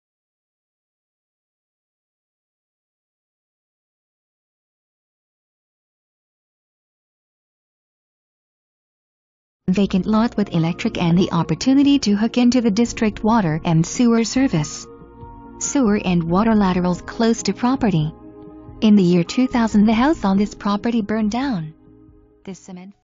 19525 Rogers Rd.
Vacant Lot with electric and the opportunity to hook into the district water and sewer service. Sewer and water laterals close to property. In the year 2000 the house on this property burned down. The cement foundation footings are still there as well as a second unit storage like structure that has electricity. Close to highway and all amenities.

Michael Bustamante BRE#01013957, Lic#01883698
Coldwell Banker Segerstrom